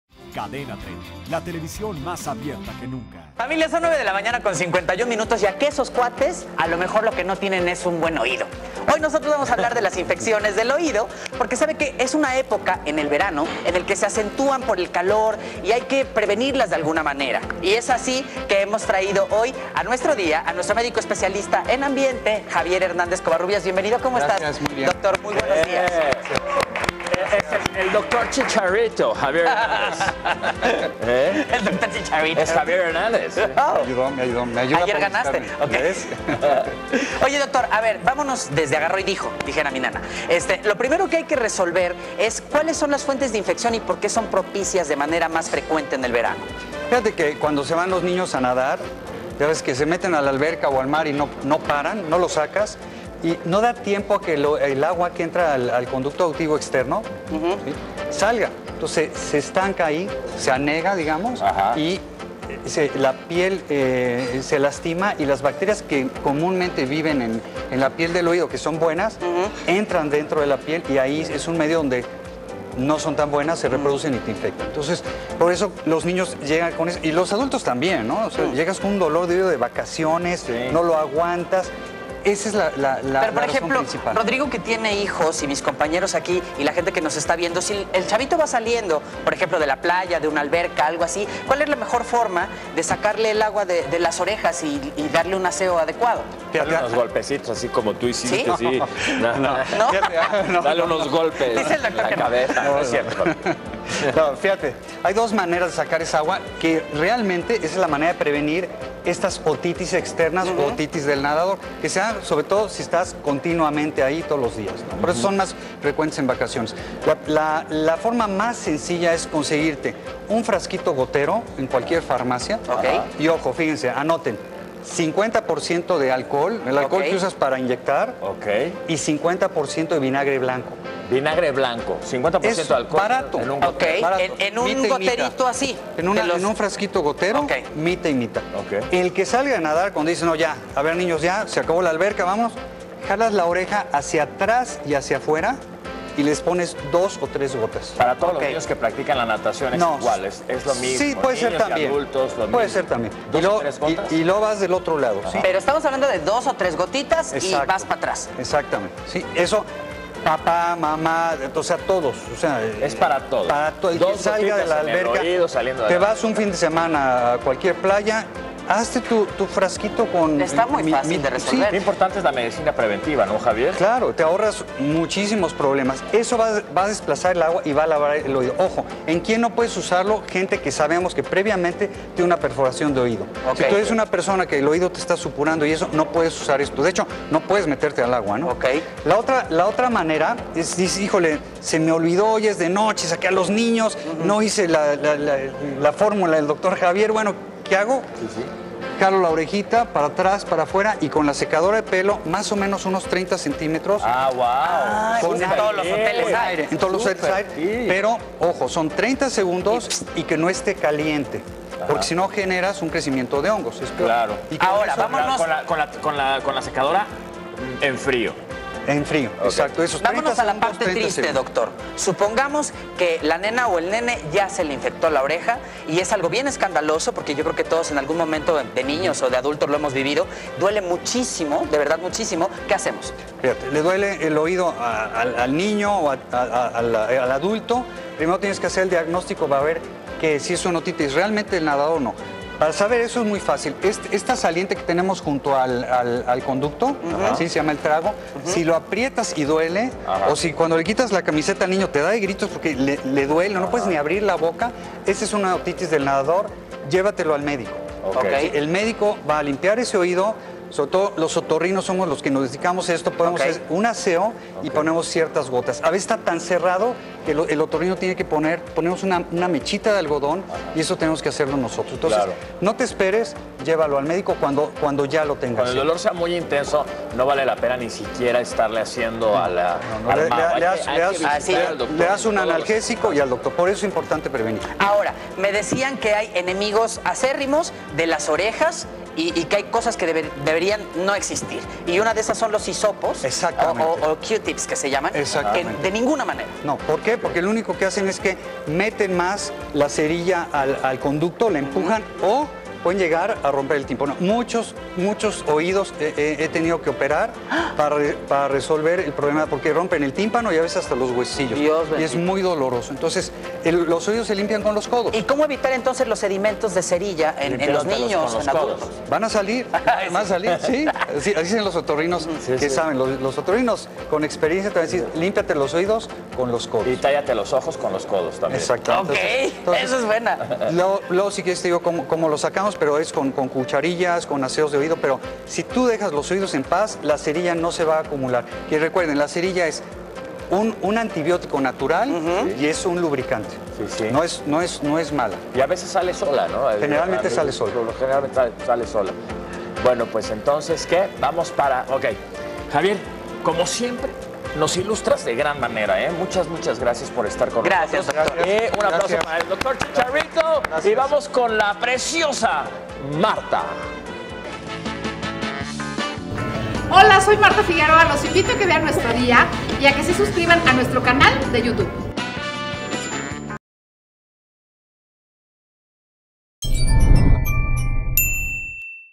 ¿Cómo prevenir las infecciones del oído?
Hoy en Nuestro Día te decimos cómo puedes prevenir las infecciones del oído.

23 julio de 2015